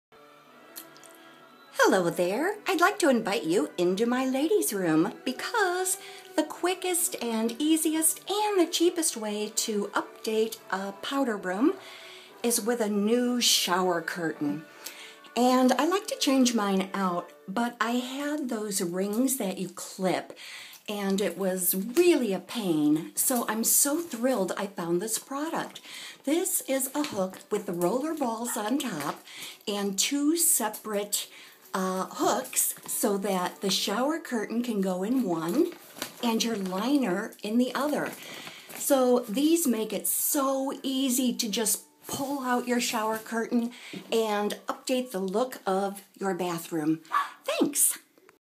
Maytex Mills Double Shower Ring
The bath is a place that is supposed to be stress free.
Climbing up on the edge of the tub and balancing like Olga Korbut while trying to un-clip those pesky plastic shower curtain rings was pretty darn stressful. And in my case, slightly dangerous, as I have the all the surefootedness of a drunken duck wearing ice skates.
Since one doesn't usually seek out curtain rings until one needs them, I had no idea a product such as this existed!

I checked out a few brands, selecting these based on price. After a month, no sign of rust, and I change curtains with ease, with decidedly more GRACE as well.
My only complaint is that the buttonhole type curtain will sometimes jump off the hook. If there is the weight of a grommet at the opening, this is not an issue. If the hook were to be just a smidge longer, these would be perfect.